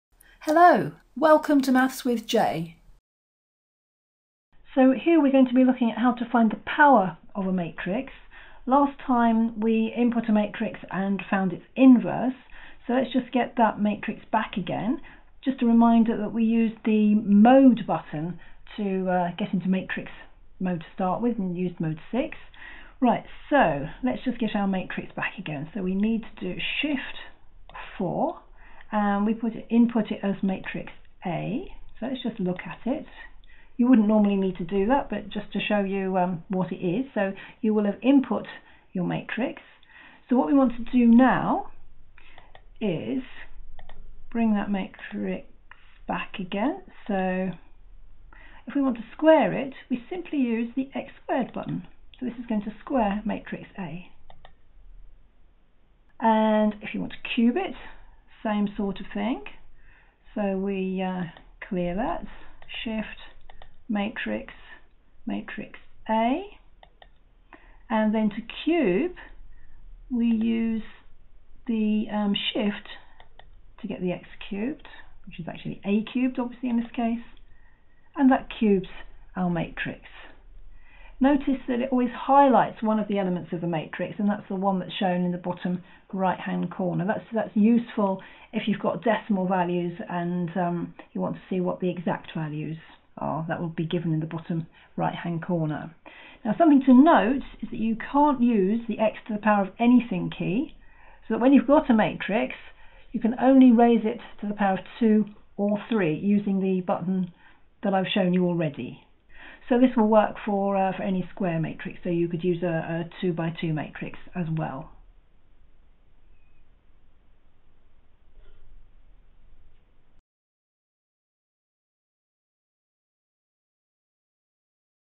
Square & Cube 3x3 matrix on Casio Calculator
How to use matrix mode to raise a square matrix to the power of 2 or 3 on Casio fx991 calculator.